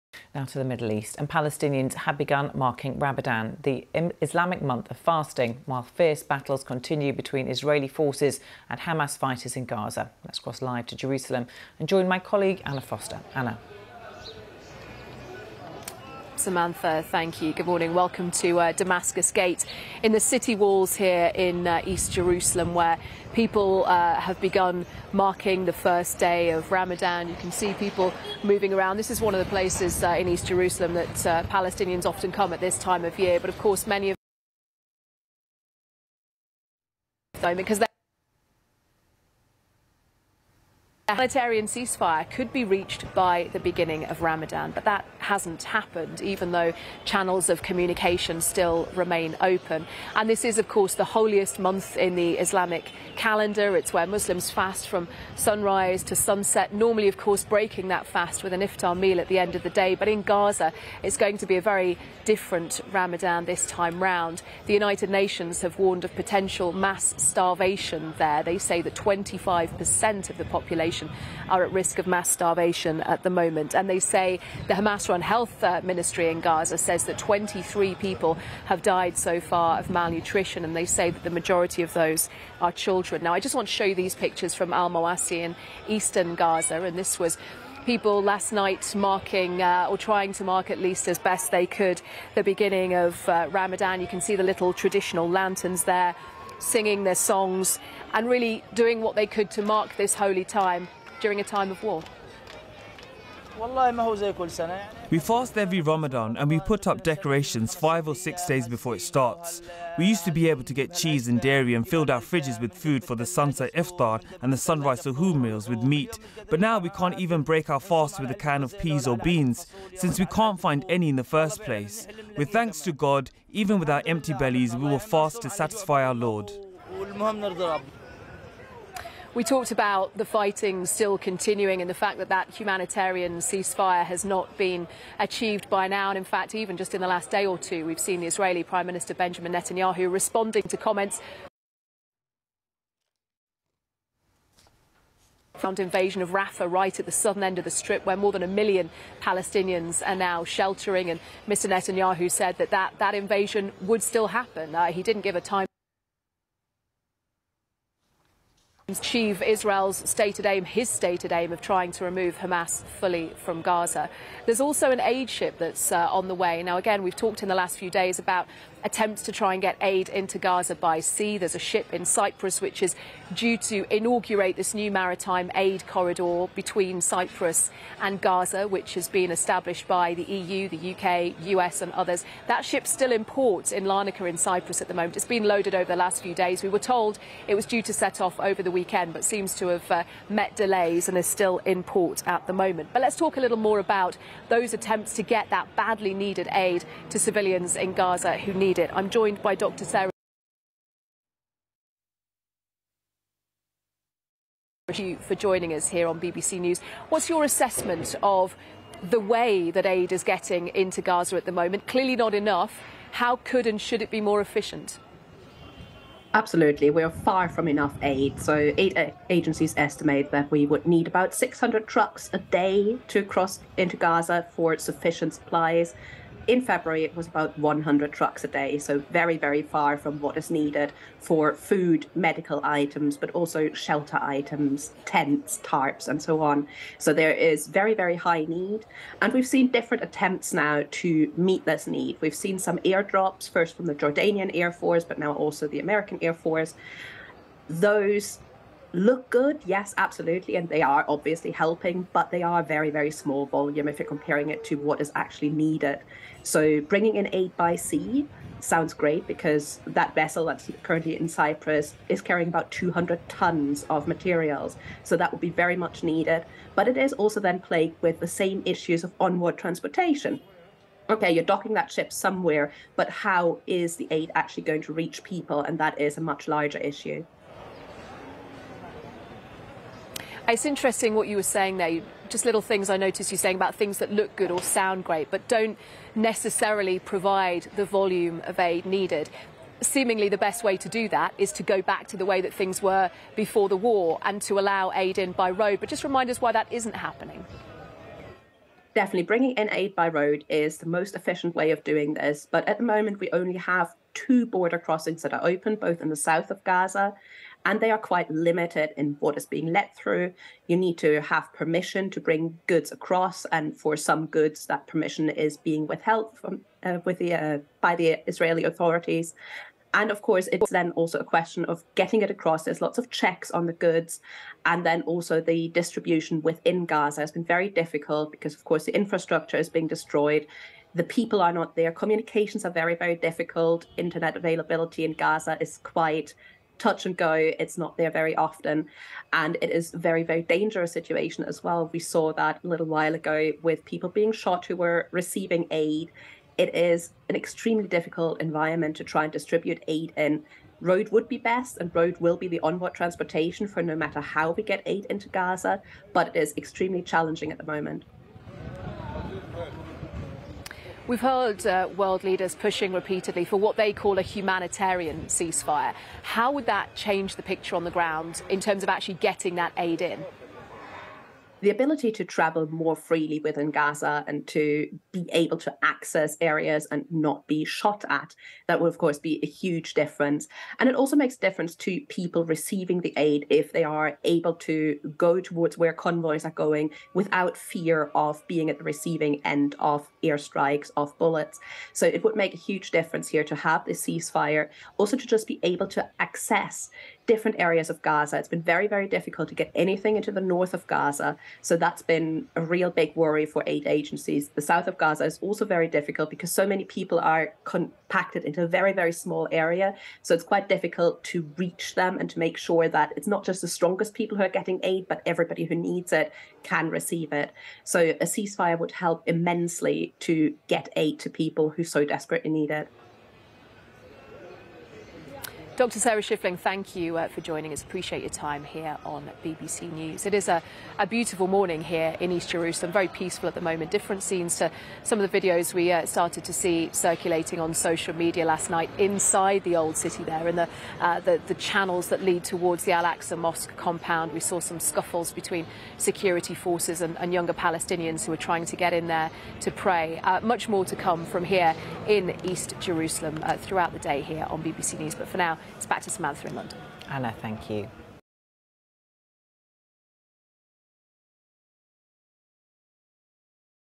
Ramadan begins amid warnings of mass starvation in Gaza-
غزہ میں بڑے پیمانے پر فاقہ کشی کے انتباہ کے درمیان رمضان کا آغاز -

Le Ramadan commence au milieu d'avertissements de famine massive à Gaza -

رمضان يبدأ وسط تحذيرات من المجاعة الجماعية في غزة -

Palestinians have begun marking Ramadan while fierce battles continue between Israeli forces and Hamas fighters in Gaza. On Sunday evening, the sighting of the new moon marked the start of the Islamic holy month of fasting. A ceasefire remains off the table as negotiations between Israel and Hamas have largely stalled. Hamas has called for Palestinians to step up visits to al-Aqsa Mosque in Jerusalem. But Israel has accused Hamas of "striving to ignite the region during Ramadan".

فلسطینیوں نے رمضان المبارک کا آغاز کر دیا ہے جب کہ غزہ میں اسرائیلی فوج اور حماس کے جنگجوؤں کے درمیان شدید لڑائی جاری ہے۔ اتوار کی شام، نیا چاند نظر آنے سے اسلامی مقدس مہینے کے روزے کا آغاز ہوا۔ اسرائیل اور حماس کے درمیان مذاکرات بڑی حد تک تعطل کا شکار ہونے کی وجہ سے جنگ بندی میز سے دور ہے۔ حماس نے فلسطینیوں سے یروشلم میں مسجد اقصیٰ کے دورے کو تیز کرنے کا مطالبہ کیا ہے۔ لیکن اسرائیل نے حماس پر الزام عائد کیا ہے کہ "رمضان کے دوران خطے میں آگ بھڑکانے کی کوشش"۔

Les Palestiniens ont commencé à célébrer le Ramadan tandis que de violents combats se poursuivent entre les forces israéliennes et les combattants du Hamas à Gaza. Dimanche soir, l'observation de la nouvelle lune a marqué le début du mois sacré de jeûne islamique. Un cessez-le-feu reste hors de question alors que les négociations entre Israël et le Hamas sont largement au point mort. Le Hamas a appelé les Palestiniens à intensifier leurs visites à la mosquée al-Aqsa à Jérusalem. Mais Israël a accusé le Hamas de « s'efforcer d'enflammer la région pendant le Ramadan ».

بدأ الفلسطينيون الاحتفال بشهر رمضان بينما تتواصل المعارك العنيفة بين القوات الإسرائيلية ومقاتلي حماس في غزة. مساء يوم الأحد، كانت رؤية الهلال الجديد بمثابة بداية شهر الصيام الإسلامي المقدس. ولا يزال وقف إطلاق النار غير مطروح على الطاولة مع تعثر المفاوضات بين إسرائيل وحماس إلى حد كبير. دعت حركة حماس الفلسطينيين إلى تكثيف زياراتهم للمسجد الأقصى في القدس. لكن إسرائيل اتهمت حماس "بالسعي لإشعال المنطقة خلال شهر رمضان".